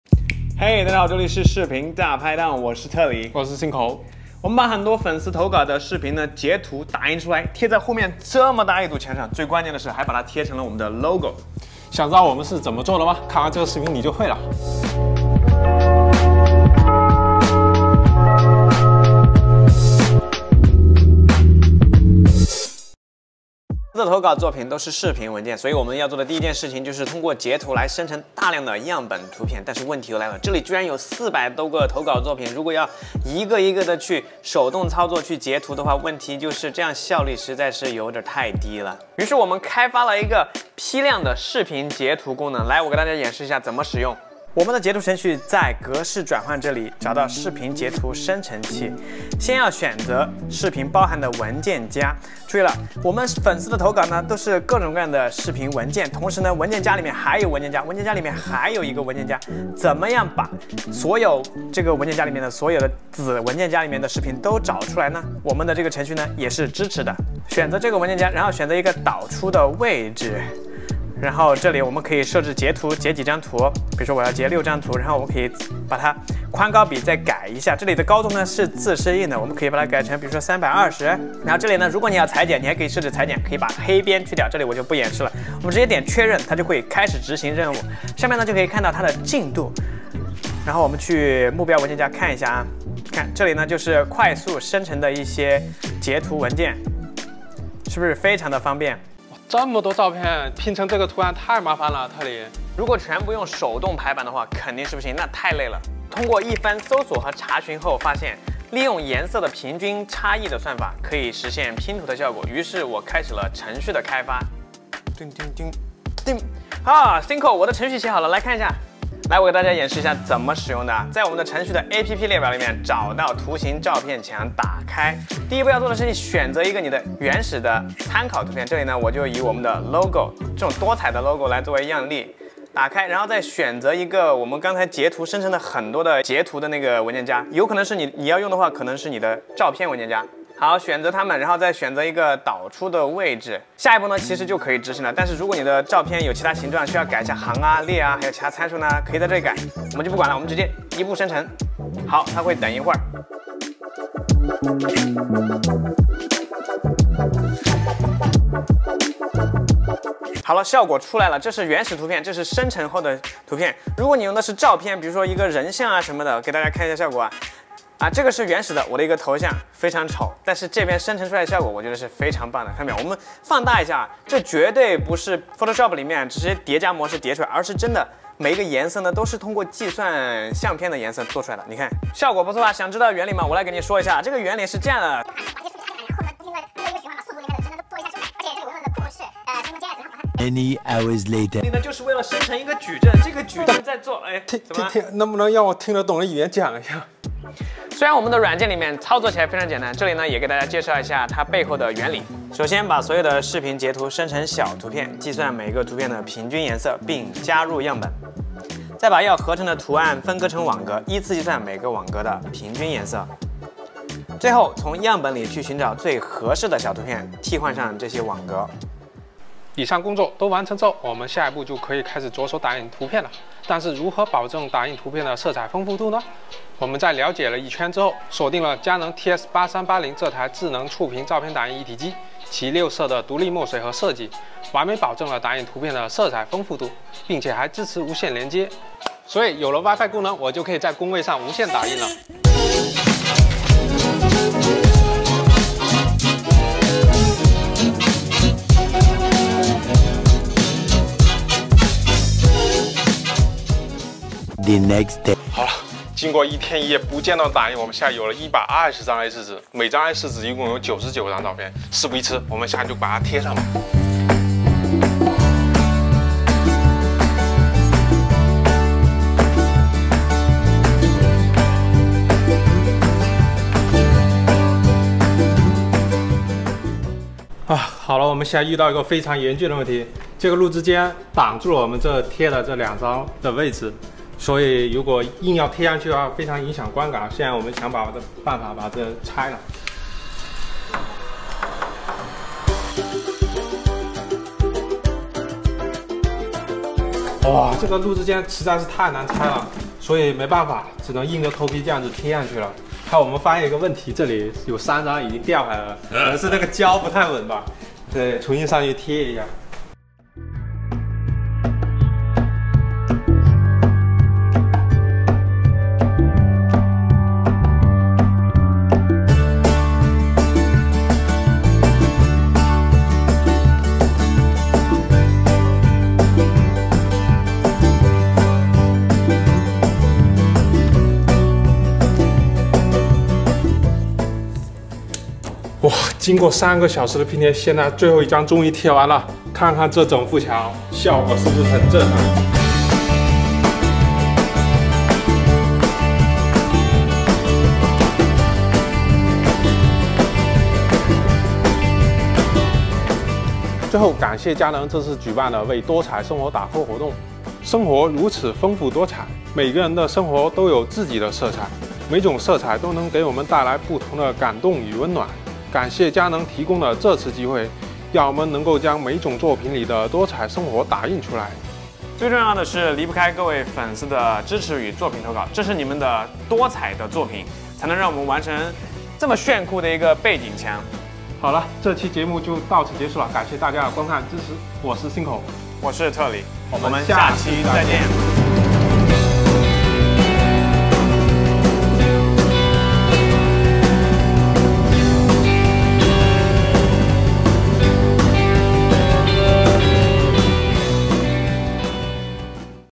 我们把粉丝的作品贴在了墙上！如何把照片库拼成另一张照片？看了这个视频就知道了
为了实现视频中演示的效果，我们开发了批量视频截图以及把大量照片拼接成另一张图的小APP，大家可以去 下载大拍档剪辑助手，免费使用这样的功能！